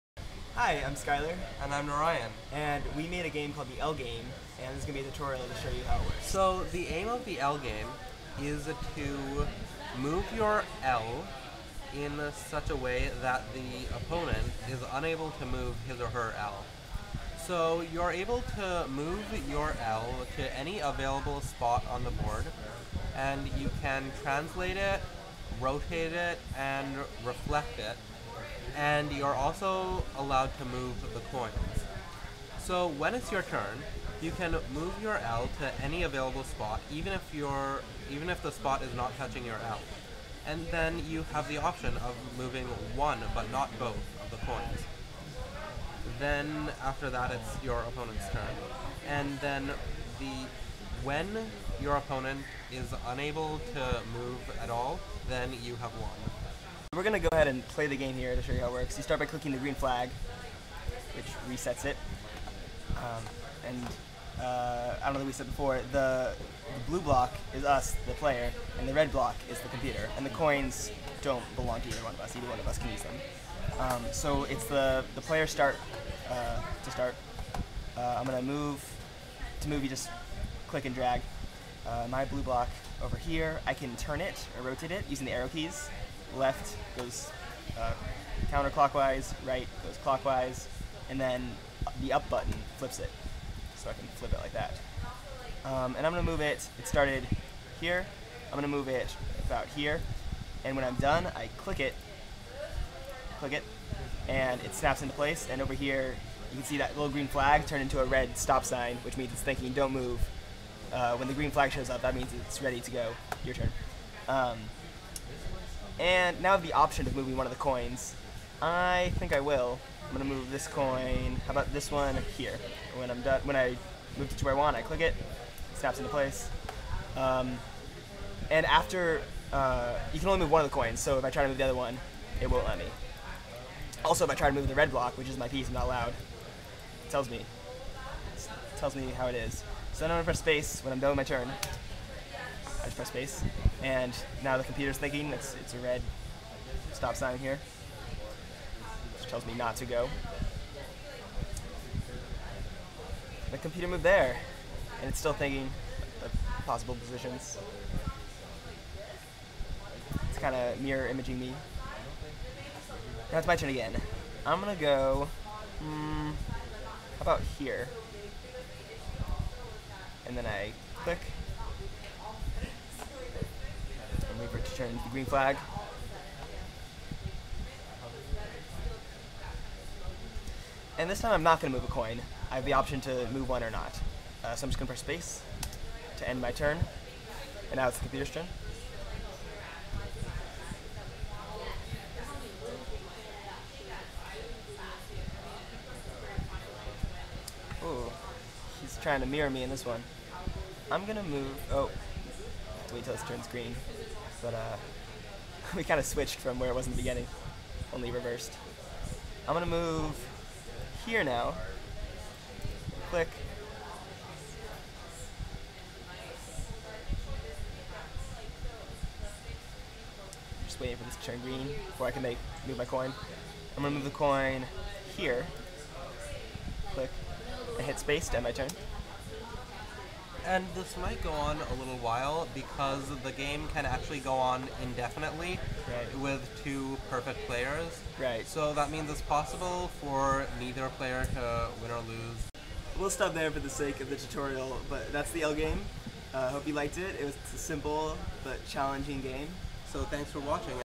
L Game Tutorial
Walkthrough of the L Game on BYOB for CS10
Made by Narayan Gopinathan and Skyler Wities